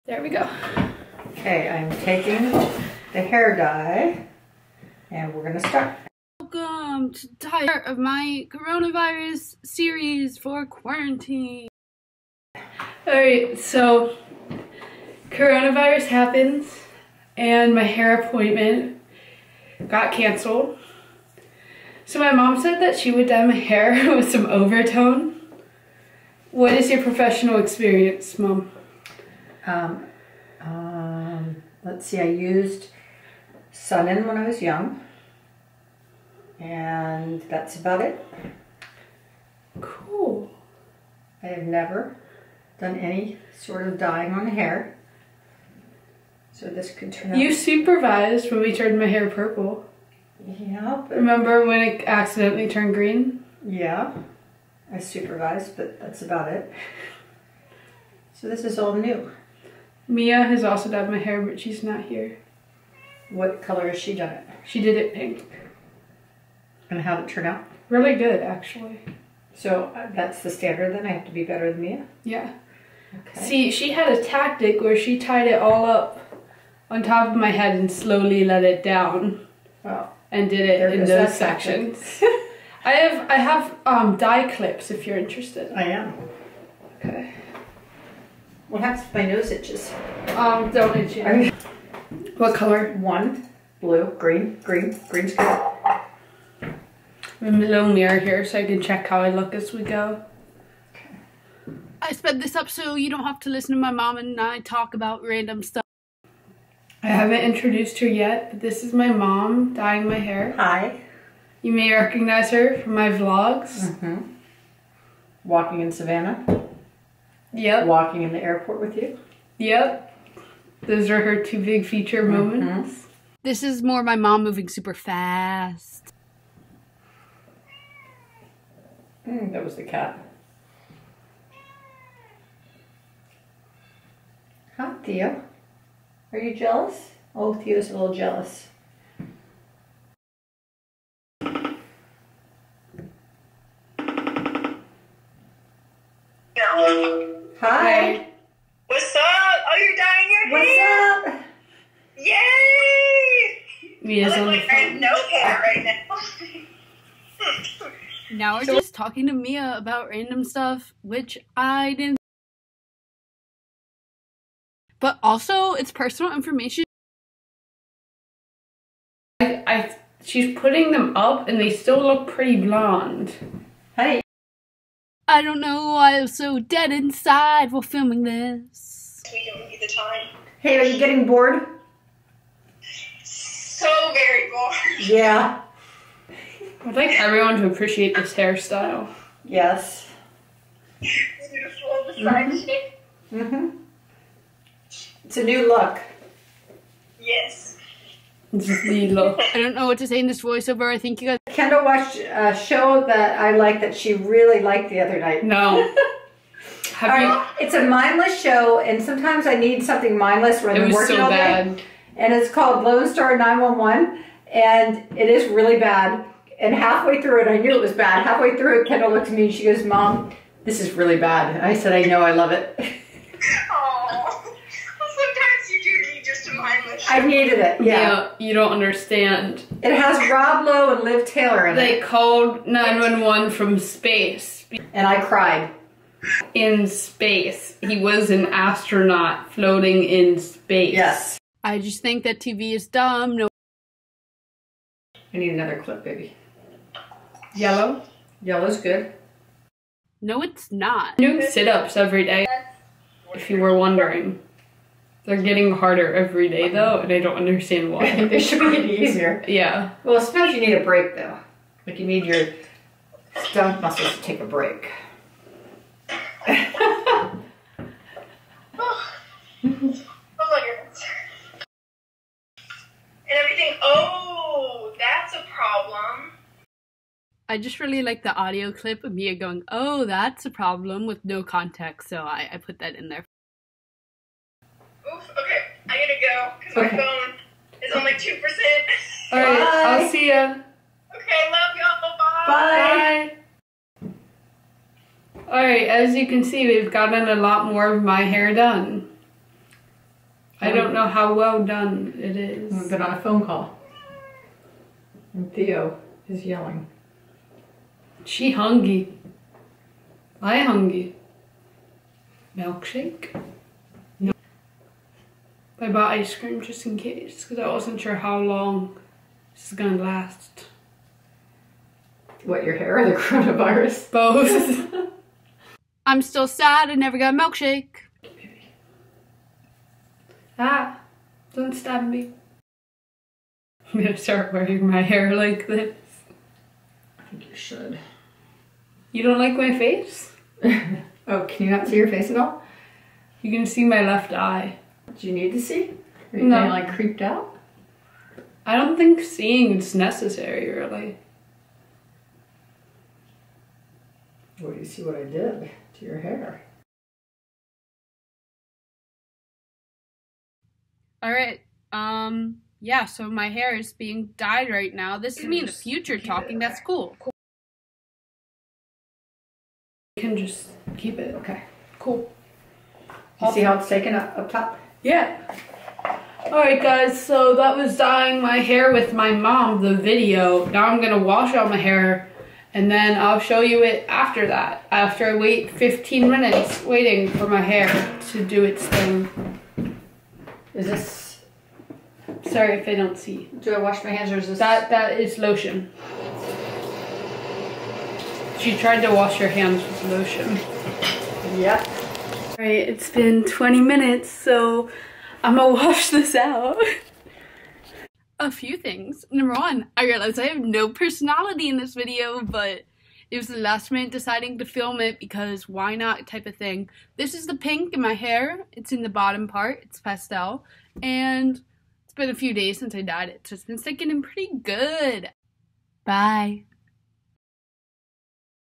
Dye my hair withme (and my mom)
Hey guys!  My hair appointment got canceled for social distancing.  As a solution, my mom decided to dye it.  That created this trainwreck of a video.